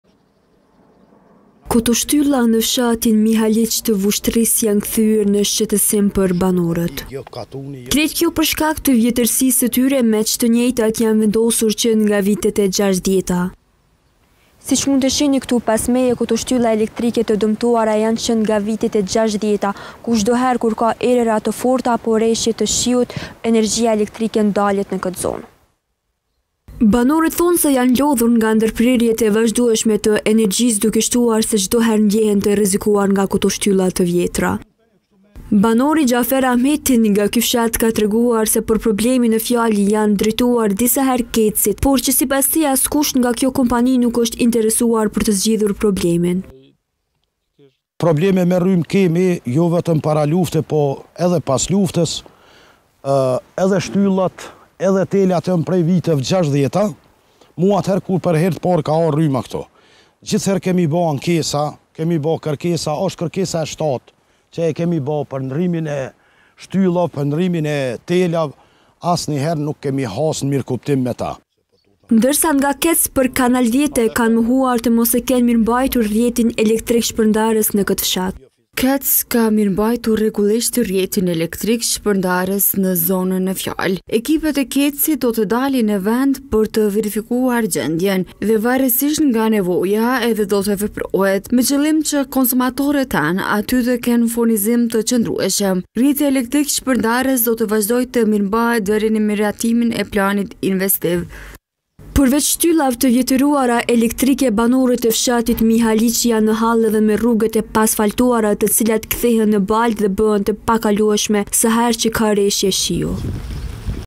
Shtyllat elektrike të viteve të 60-ta, shqetësim për banorët e Mihaliqit
🔴 Raport - Ushtria e Serbisë afër Kosovës, “alarmon” Kurti - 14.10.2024